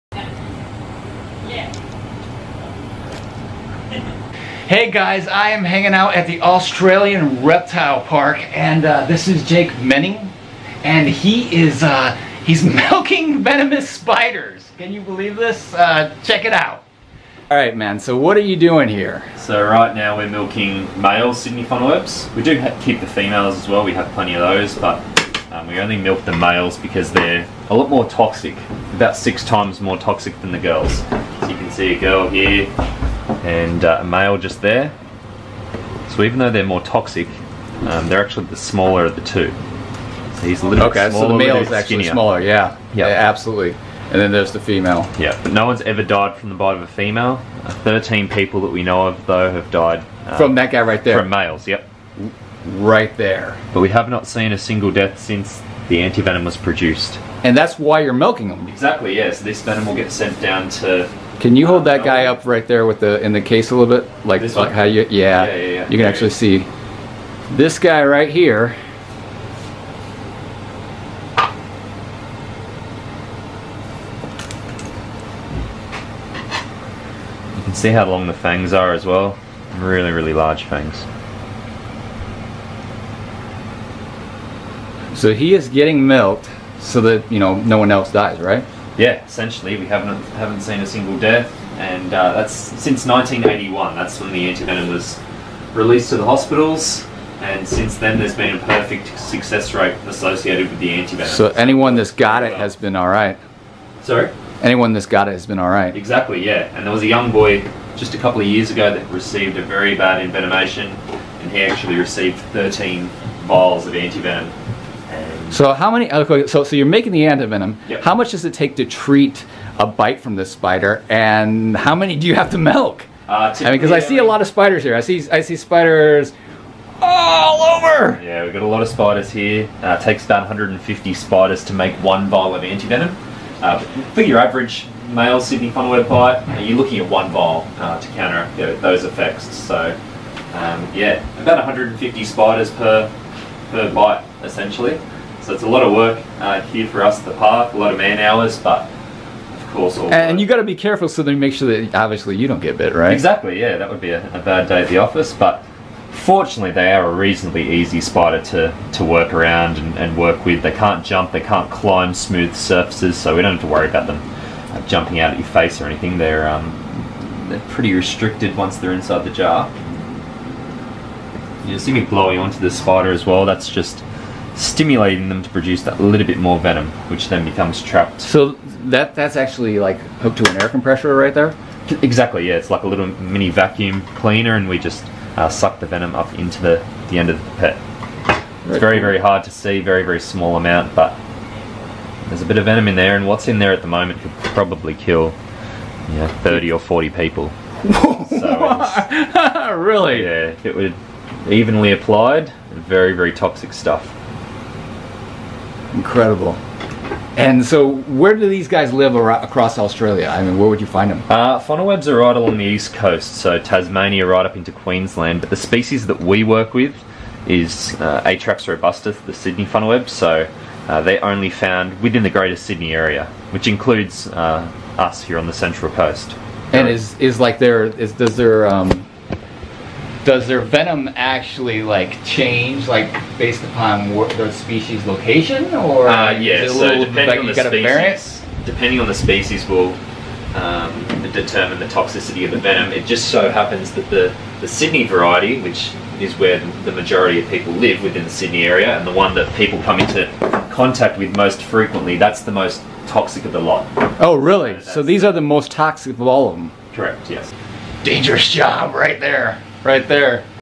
Most Dangerous Job? Milking Venomous Spiders to Save Lives!
Have you ever wondered how antivenom is made? 🤯 This video takes you behind the scenes at the Australian Reptile Park to see how they milk venom from one of the world's most dangerous spiders - the Sydney Funnel-Web! 🕷️